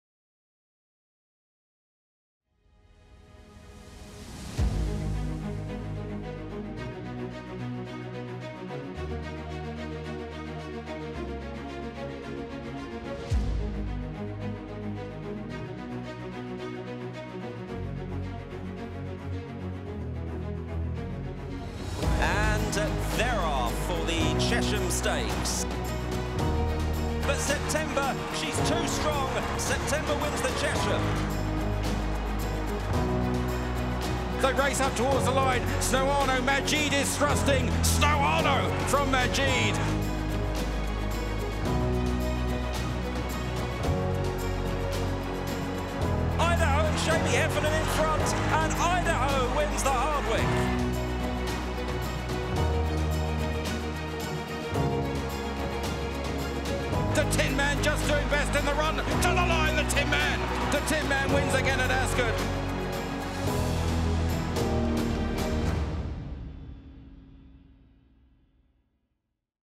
Royal Ascot 2017 | Day Five Highlights
As the Royal Meeting comes to a close, relive the highlights from day five as The Tin Man wins the Diamond Jubilee Stakes.